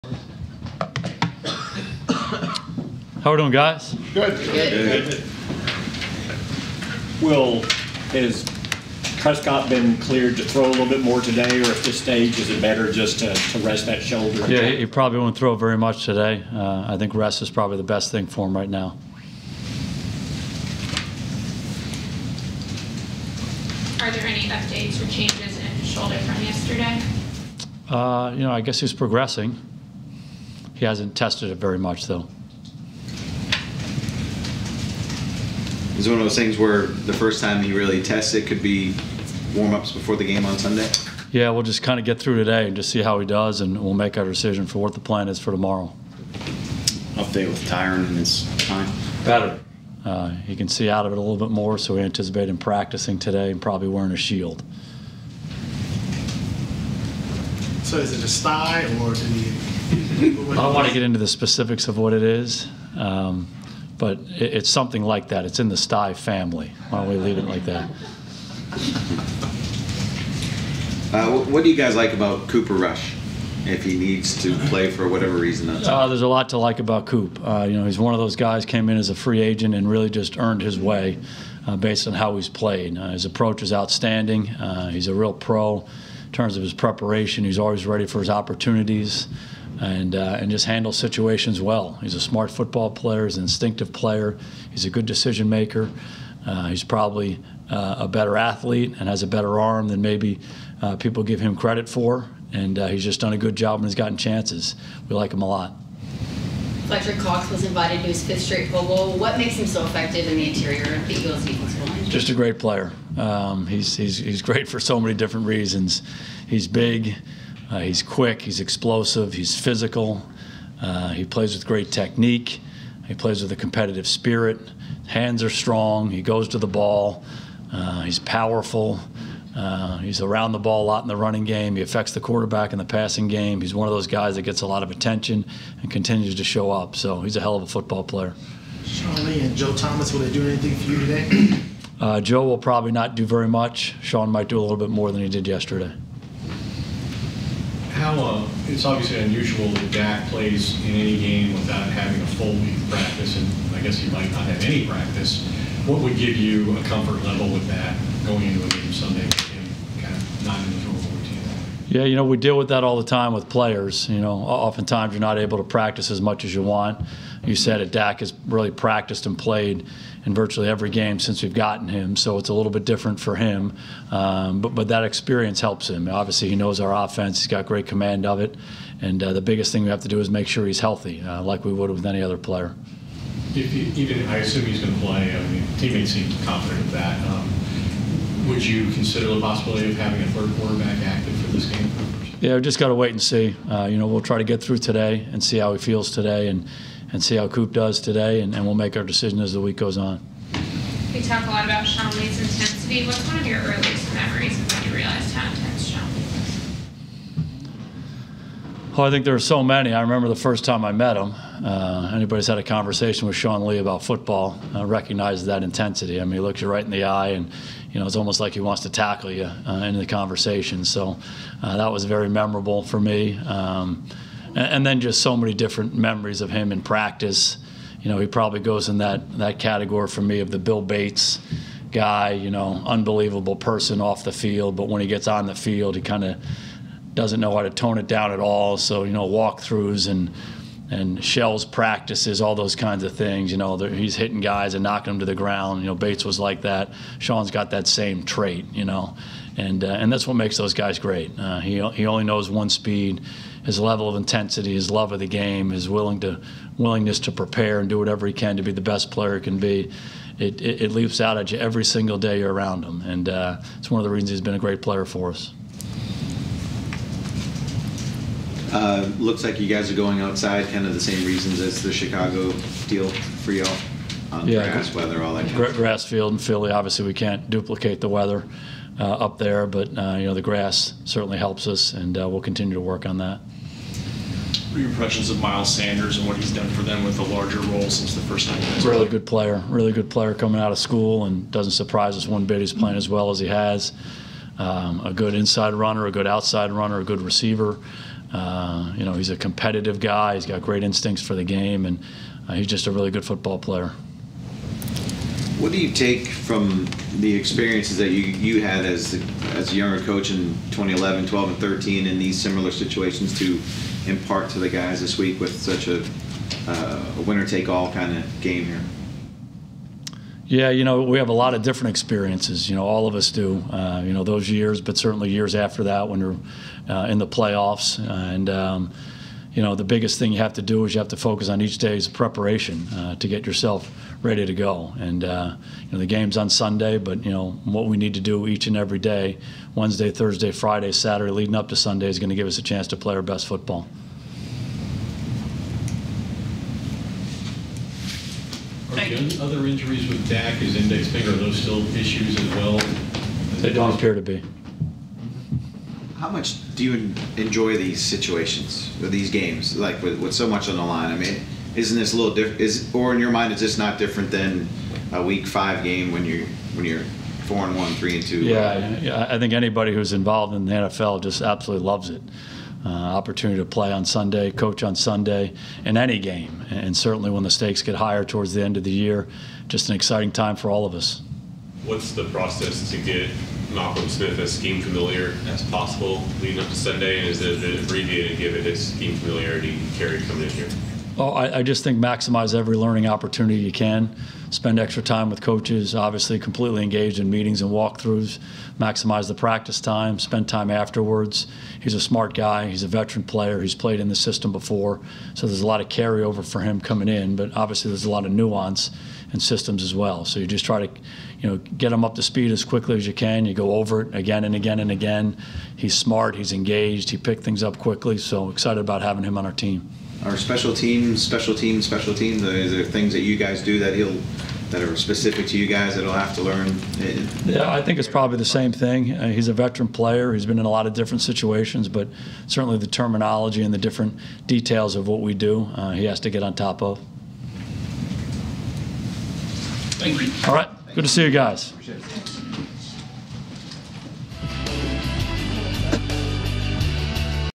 Jason Garrett: Dak Prescott Injury Update; More | Dallas Cowboys 2019
Head coach Jason Garrett updates the injury status of the QB Dak Prescott, shares his thoughts on backup QB Cooper Rush and more.